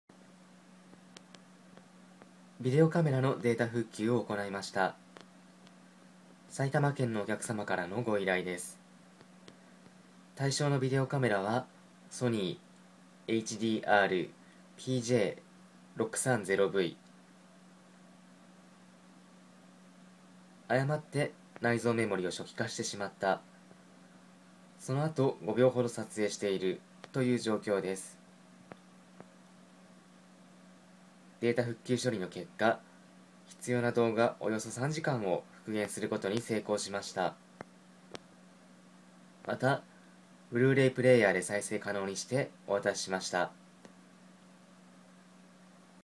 内蔵メモリを初期化したビデオカメラからデータ復旧に成功 SONY HDR-PJ630V
内蔵メモリを初期化したSONY HDR-PJ630V ビデオカメラからデータ復旧に成功しました。
ビデオカメラのデータ復旧はお任せ下さい。